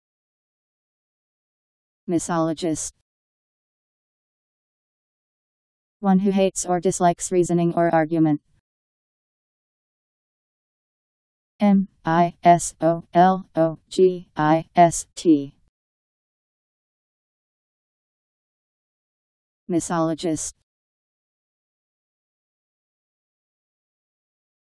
Misologist Meaning
Video shows what misologist means. One who hates or dislikes reasoning or argument..  Misologist Meaning. How to pronounce, definition audio dictionary. How to say misologist. Powered by MaryTTS, Wiktionary